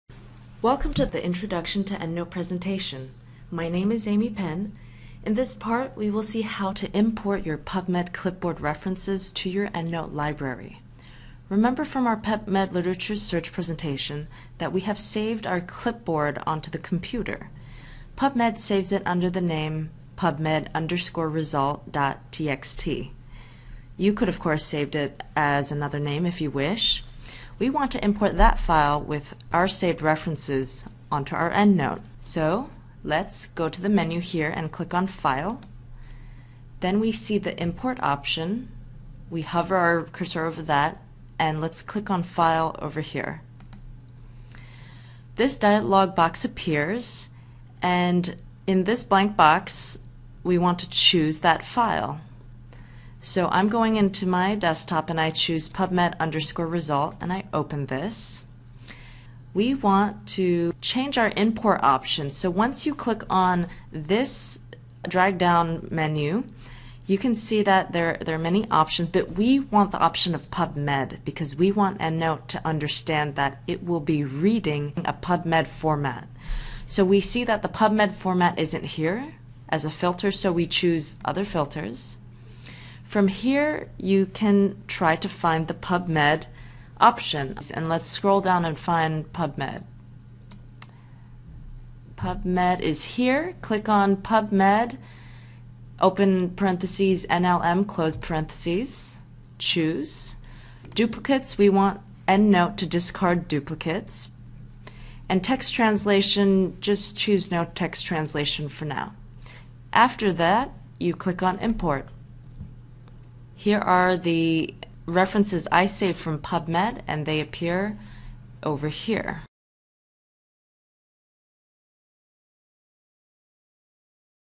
Introduction to EndNote - Importing References from PubMed
Learn how to import references from PubMed into EndNote